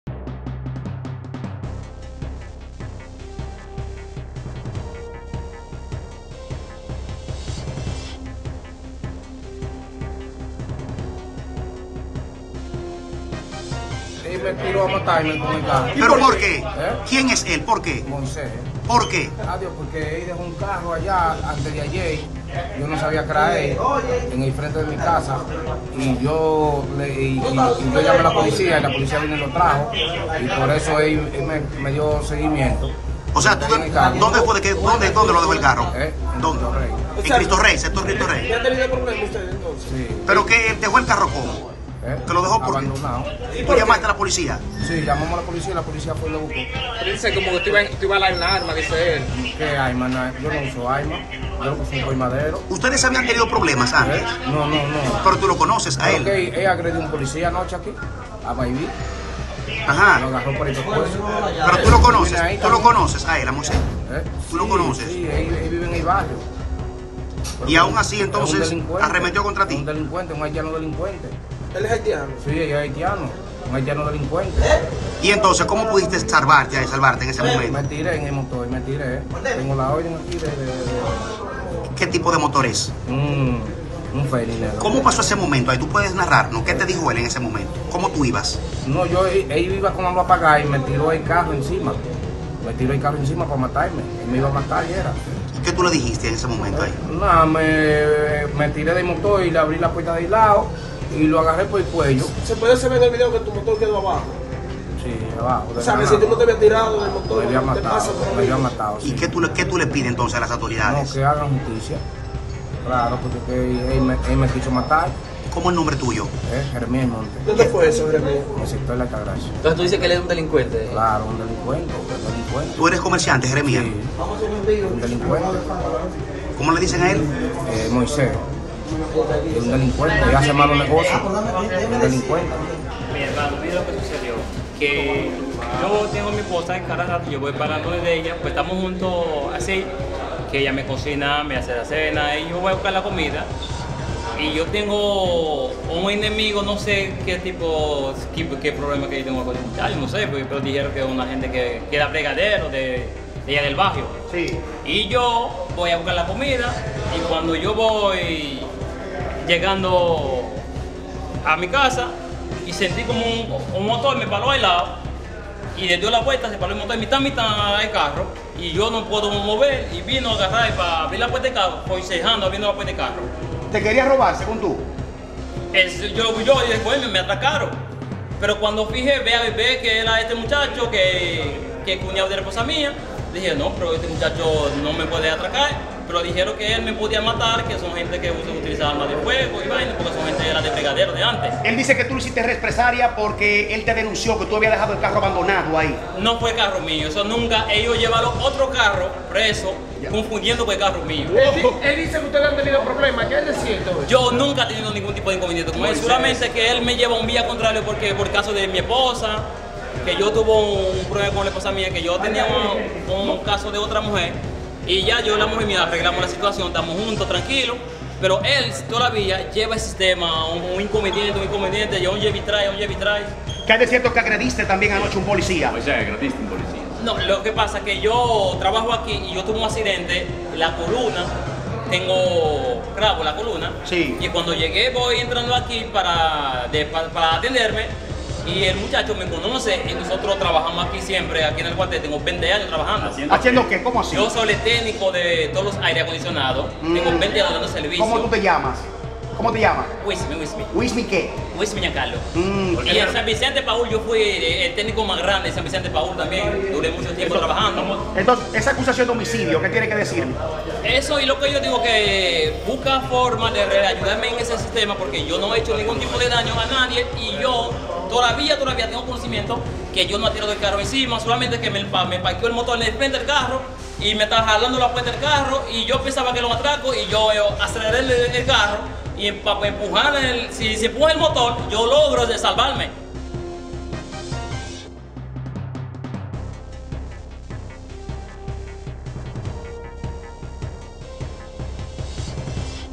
Hombre denuncia que un haitiano lo atropello en SFM con intención de matarlo
San Francisco de Macorís. – Un hombre acudió este jueves a la Dirección Nordeste de la Policía de esta demarcación, el mismo denuncio que un individuo identificado como Moisés de nacionalidad haitiana, supuestamente intentó asesinarlo al atropellarlo con un carro en el sector Cristo Rey de esta ciudad.

Según el denunciante, el conflicto se desató luego de que el tal Moisés dejara abandonado un vehículo frente a su vivienda, y en ese momento cuando observó el carro estacionado por un largo periodo el denunciante decidió llamar a la Policía, quienes se llevaron el automóvil.